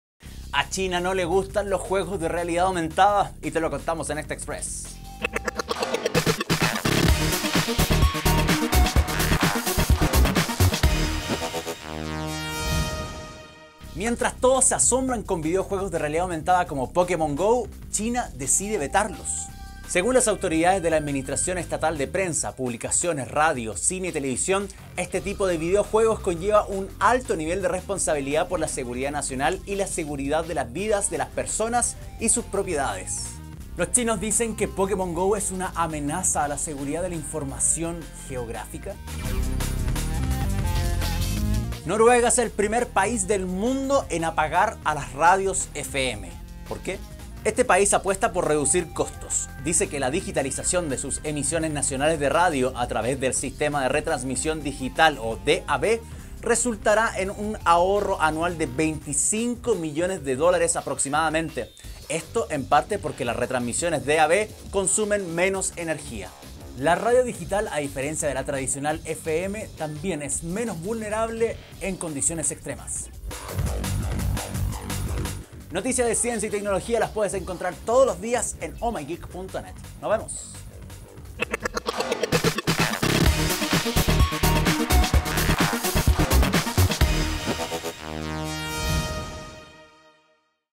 OMG! Express: Noruega pone fin a las radios FM y China en contra de Pokémon Go
En este capítulo del Express te contamos la decisión de Noruega que transformó todas sus estaciones de Radio FM al sistema digital DAB, eliminando la frecuencia modulada.

Además, China tiene una relación de odio con los videojuegos de realidad aumentada porque representan una amenaza para quienes lo juegan y los ciudadanos. ¿En serio? Sí, también lo descubrirás en este episodio.